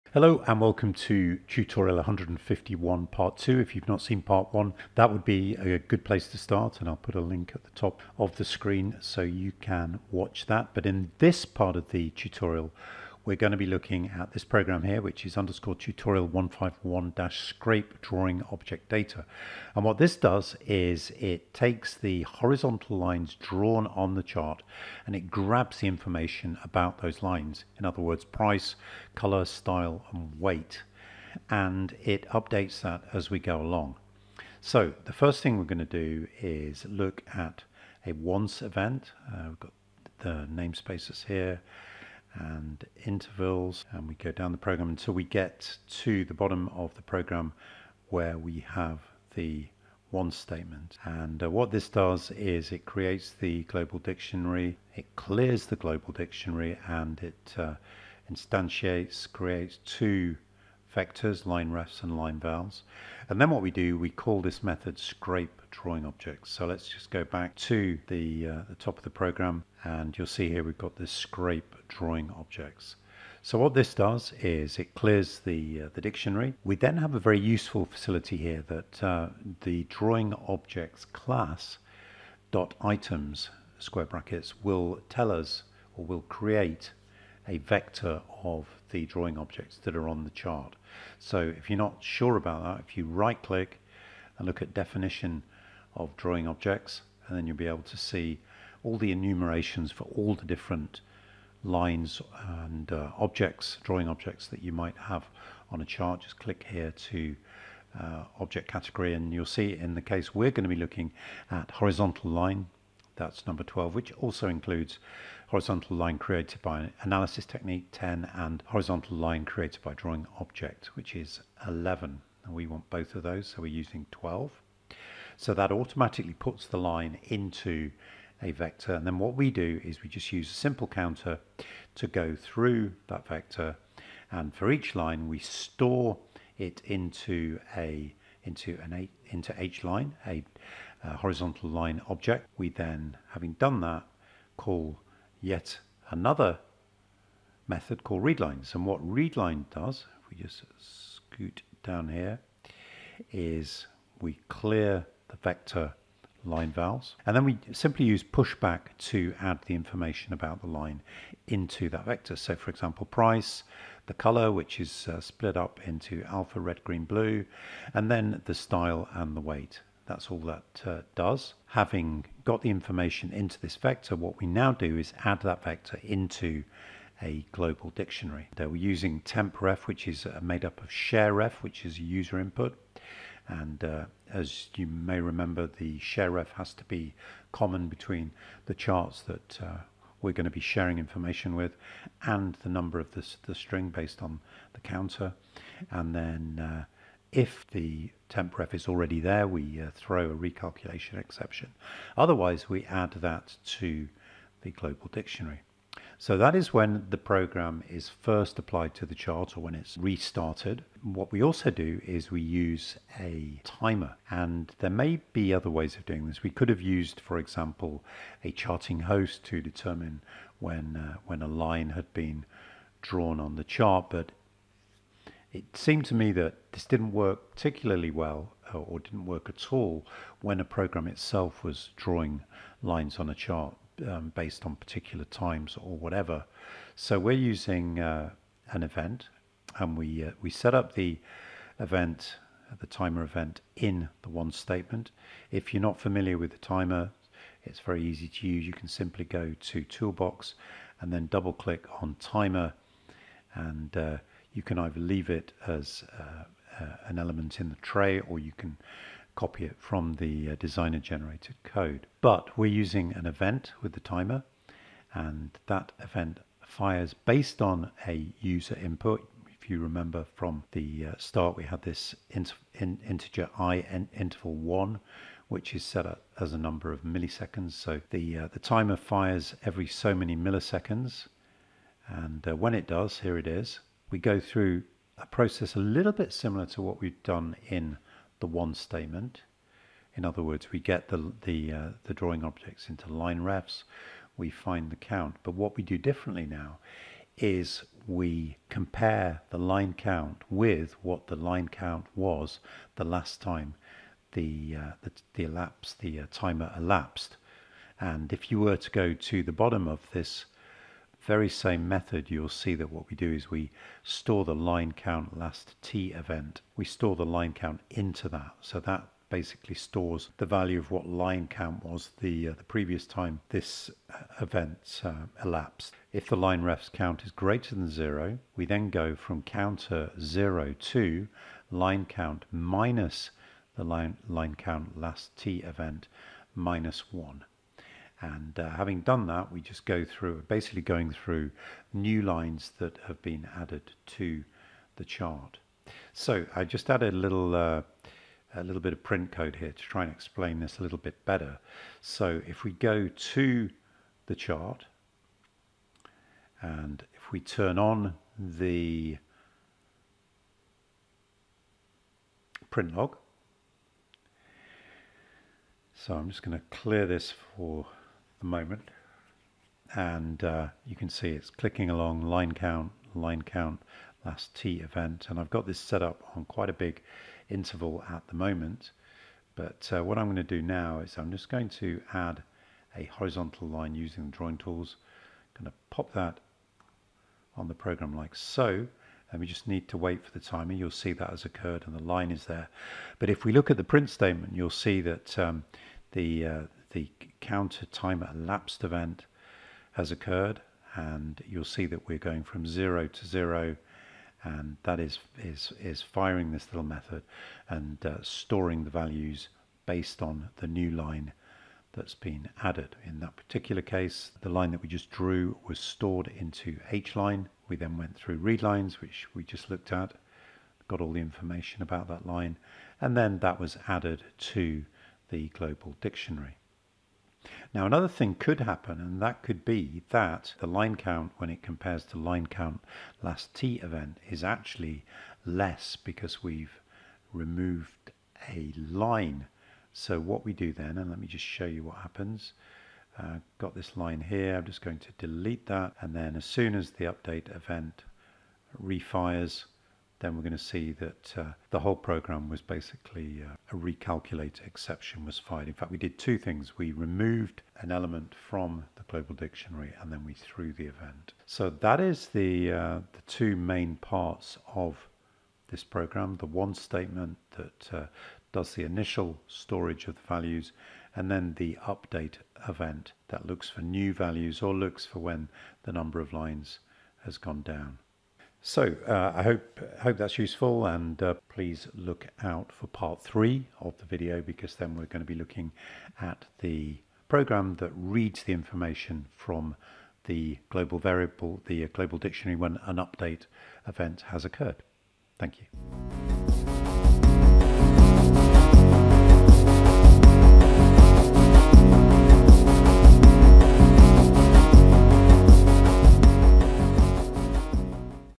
TradeStation Tutorial 151-Syncing lines between two charts using the Global Dictionary Part 2
Tutorial 151 demonstrates how to create TradeStation EasyLanguage programs to sync lines between two charts using the Global Dictionary. The technique could be useful, for example, if you were using a program that was protected (i.e. you cannot see the code or modify it) that produced drawing objects on a chart. You might wish to replicate those drawing objects on another chart.

Tutorial 151 shows a way of doing this using horizontal lines drawn by an example program (included in the download).

Part two includes a description of _Tutorial151-ScrapeDrawingObjectData, the program that 'scrapes' horizontal line data from a chart and stores it isn Global Dictionary.